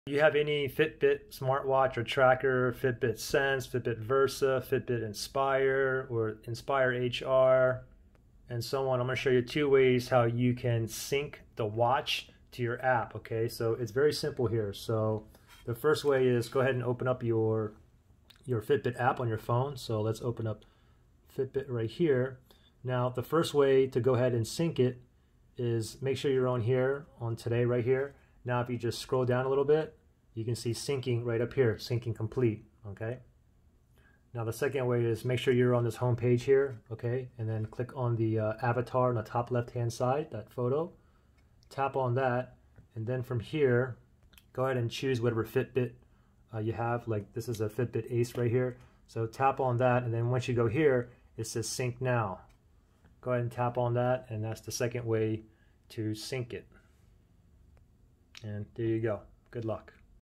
All Fitbits: 2 Ways to Sync to Fitbit App
I show you 2 ways on how to unpair any Fitbit watch/tracker/smartwatch from the Fitbit app on your phone.

In this video i am using a Fitbit Sense